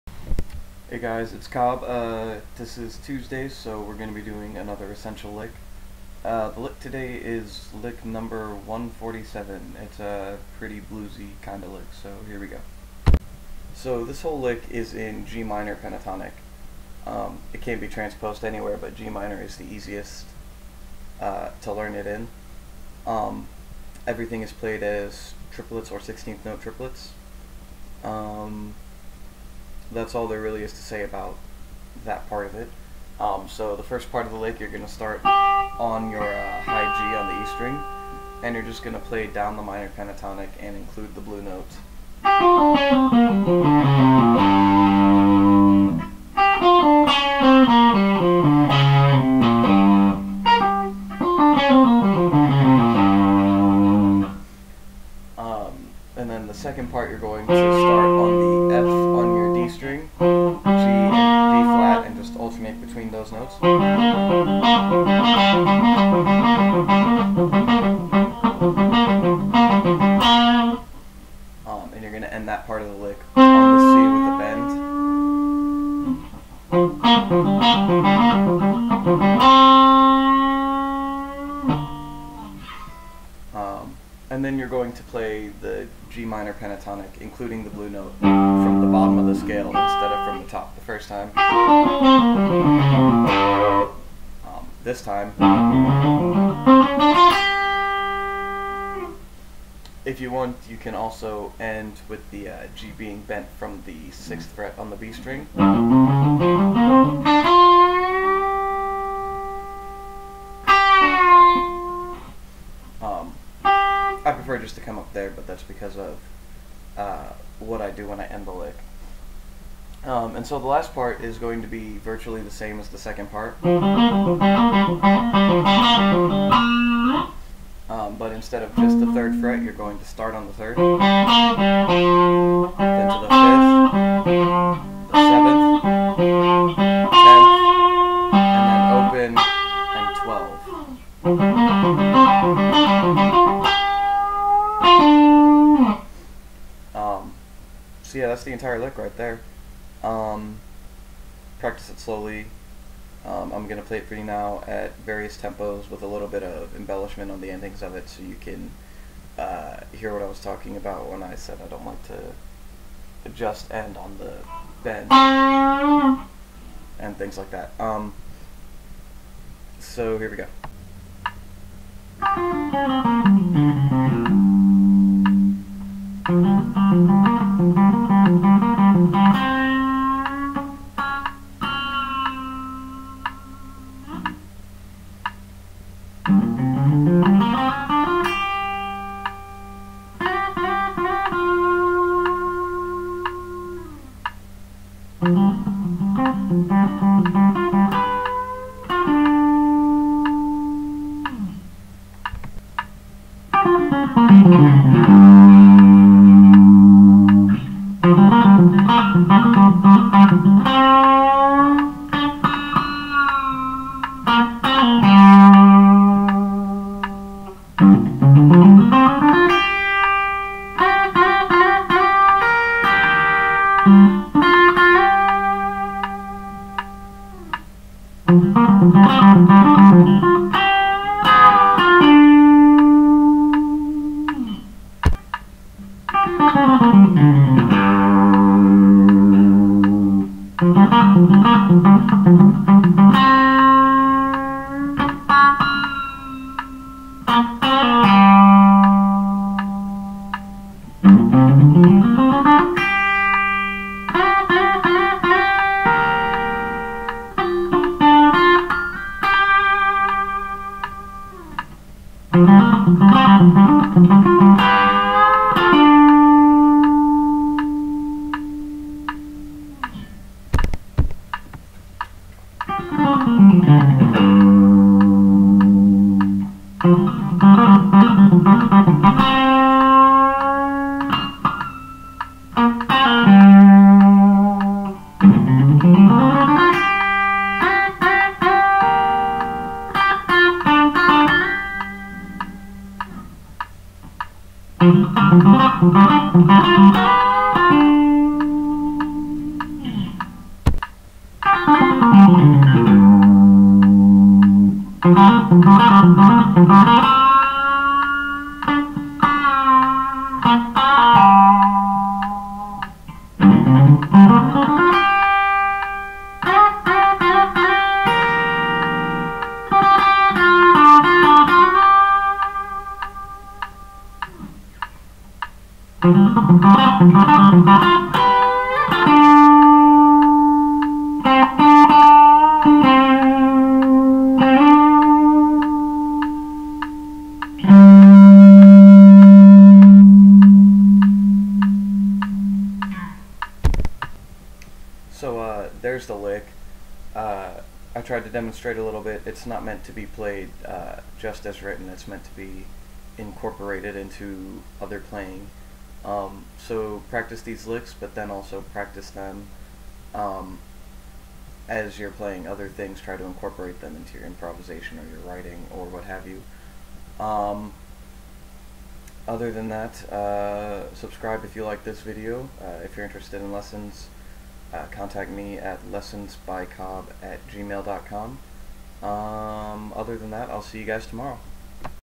Essential Lick number 147
Essential Lick Number 147!  This is a Gmin pentatonic lick, very well suited for blues.  It's actually a few different licks played right after each other.  For the purpose of learning, you can play it this way but, each section of the overall lick is well suited to be played and incorporated into your solos in its own way.  If you can, string them together with some good musical phrases between them. I show a little  bit of this  but, just enough to communicate that you aren't stuck to what's written when playing this lick, as you are with some others.

I'm a bit sick today so, forgive the eye rubbing, yawning, or anything else, pee el zee